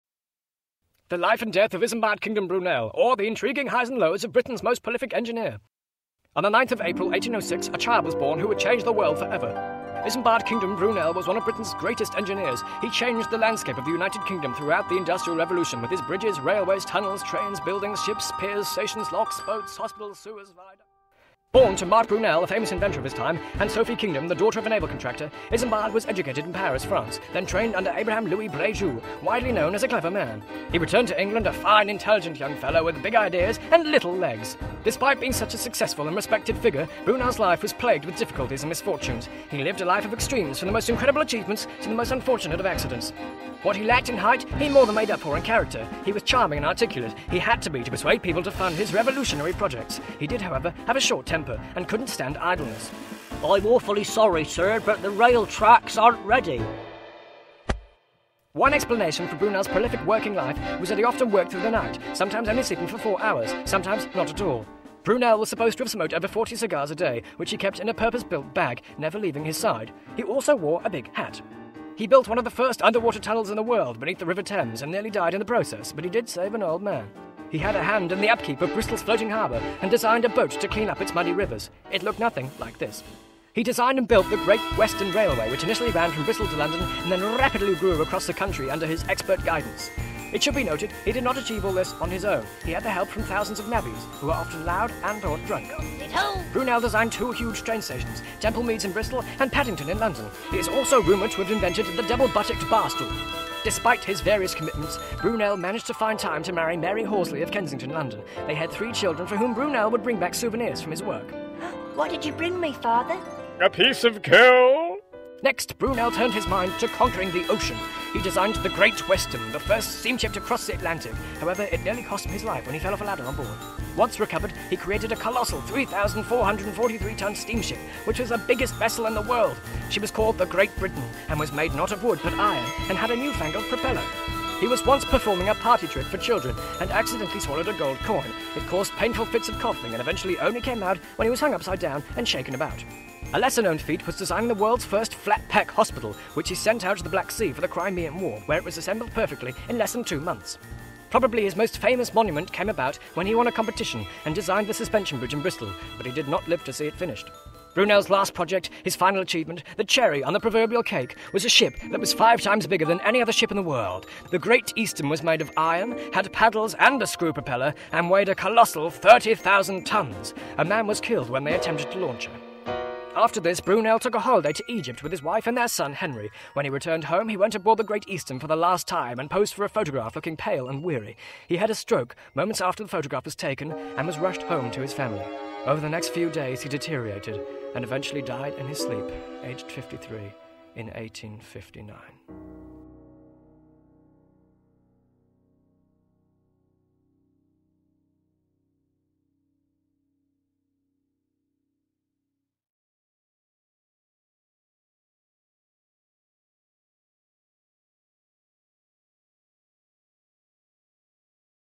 The Life and Death of Isambard Kingdom Brunel
An animated documentary about the British engineer Isambard Kingdom Brunel.
Written, Animated and Voiced by Joseph Wallace.
Music Composed by Theo Jamieson.